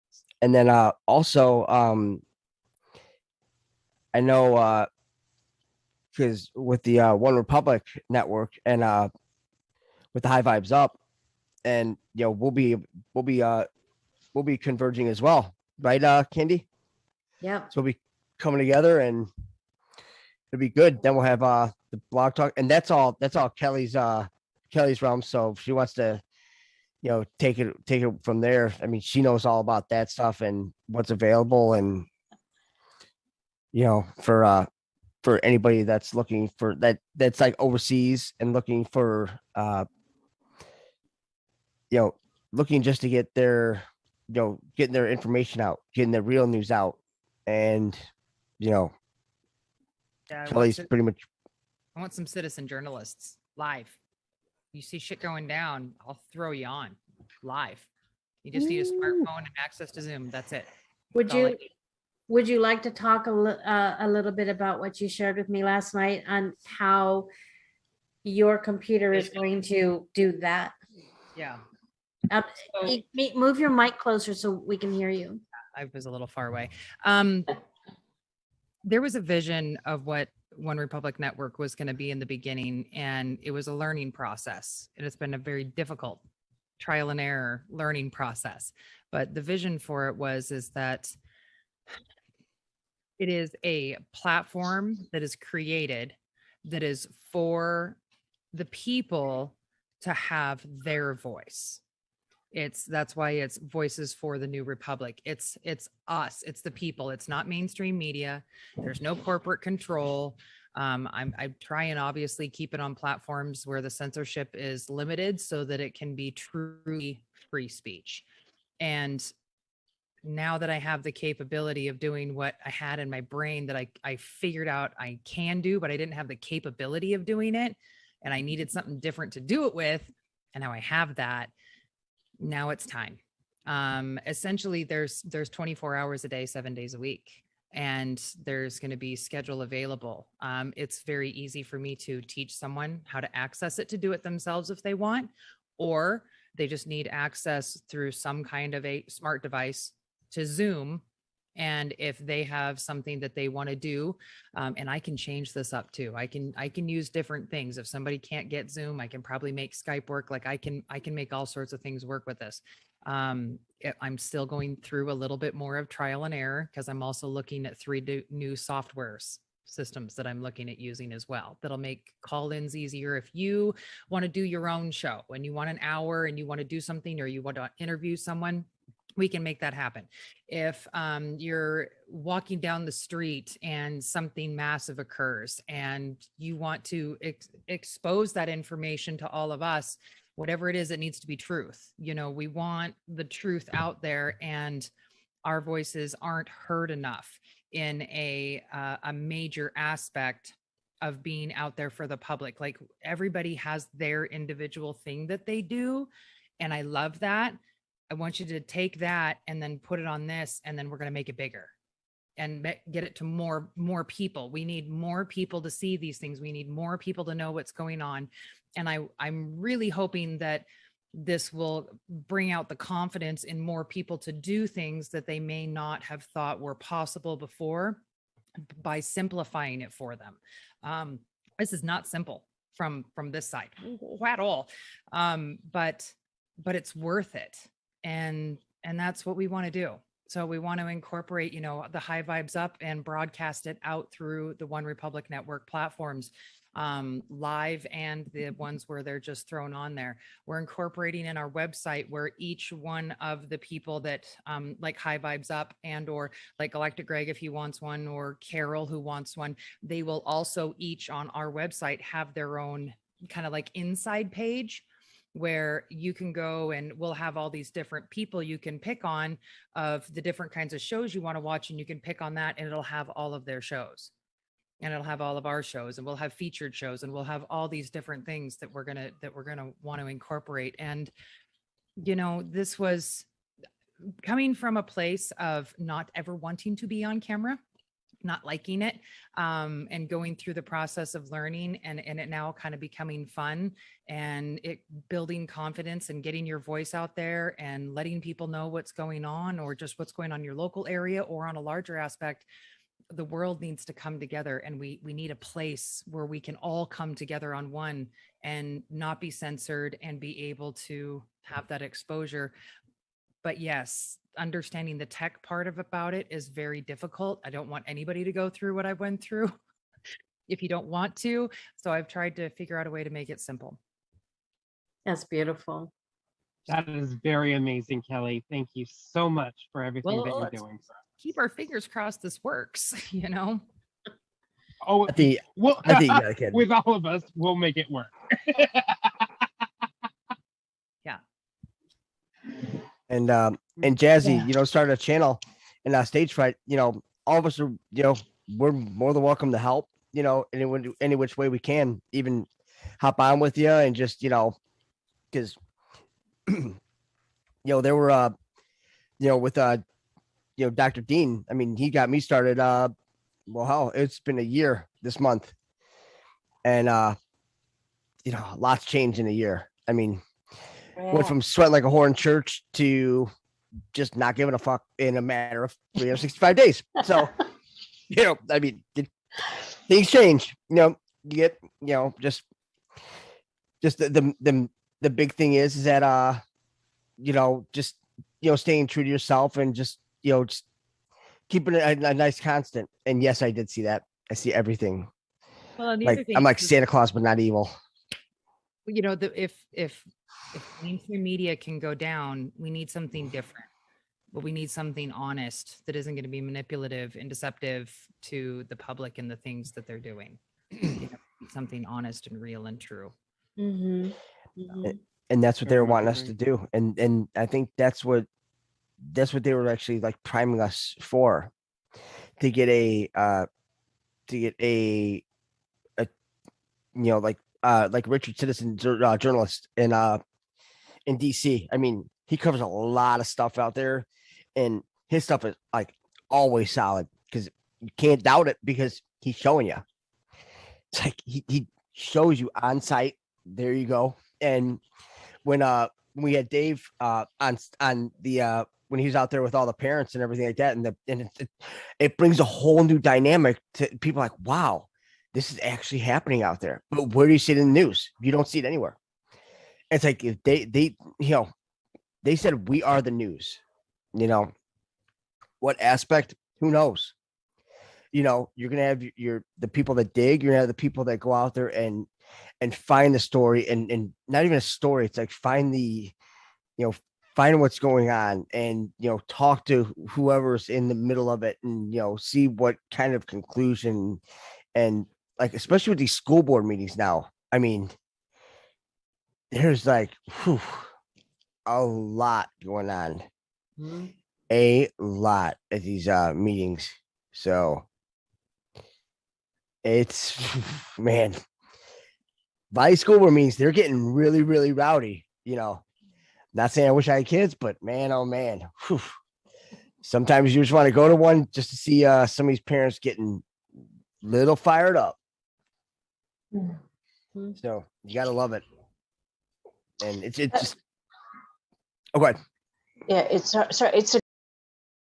WTF-About? ORN and HVU...Got something up there Sleeve. Ova
What To Feel About?

ORN and HVU. Got something up there sleeve.

Kelli Discusses why ORN was Started and the Future they are looking forward too.

Peace, Light, Love and Happiness in Abundance to All Beings of Our Mother Earth Plain. Ova